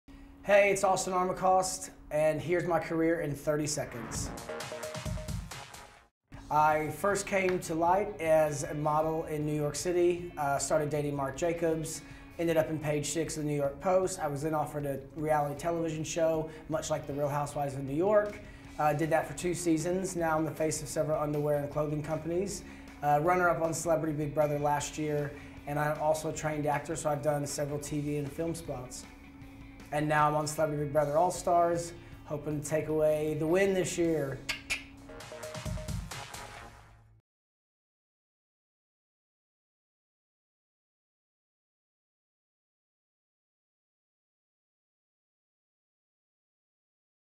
My Career in 30 Seconds | Austin Armacost
Here is a quick guide to Austin's career highlights in 30 seconds. Sit back and enjoy!